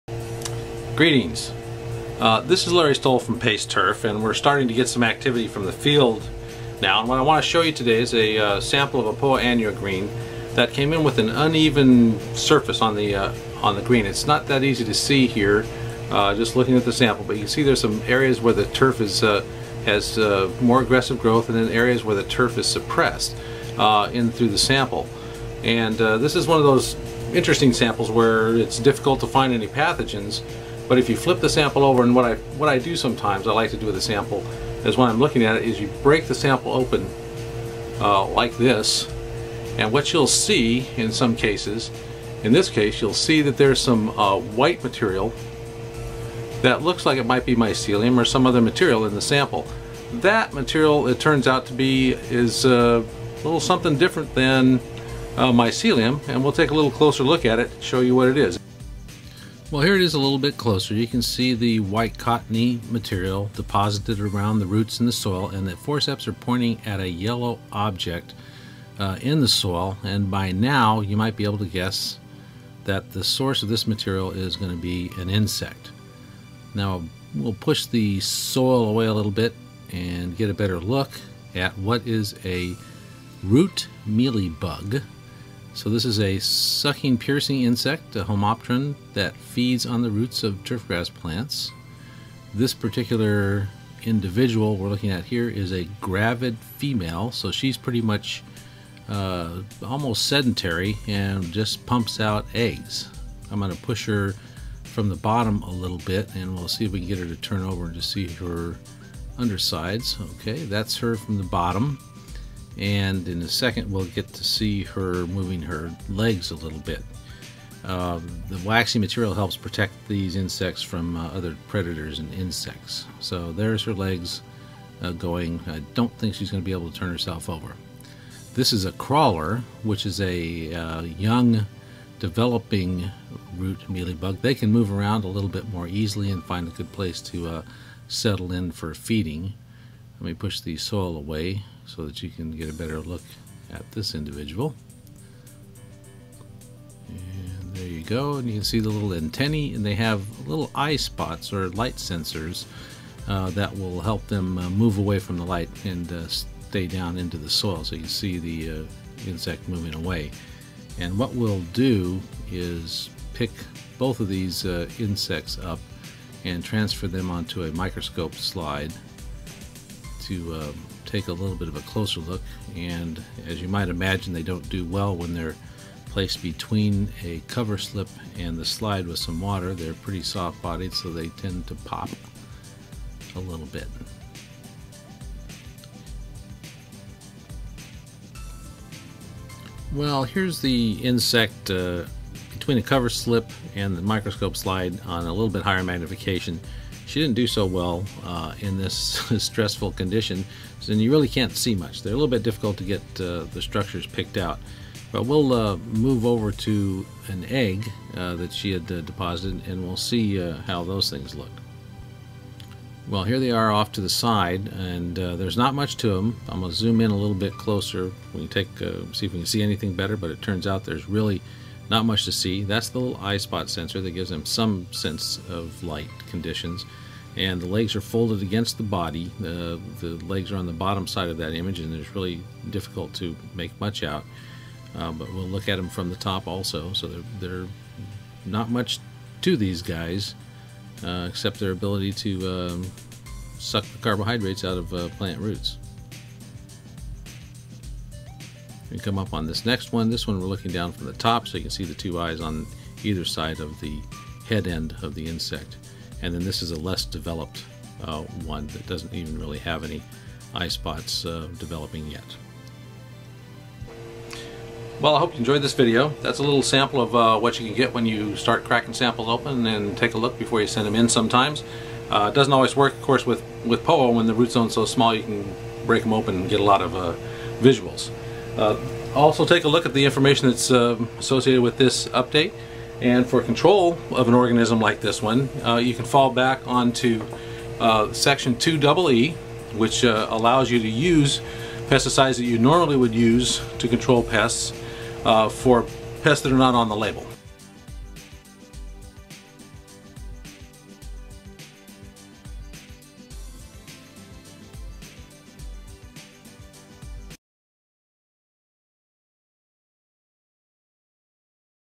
Root Mealy Bug
These soil dwelling mealybugs occur on the roots of several different turf types (bluegrass, bermudagrass, bentgrass, kikuyugrass), where they are frequently associated with a bleaching of turf foliage known as "white leaf".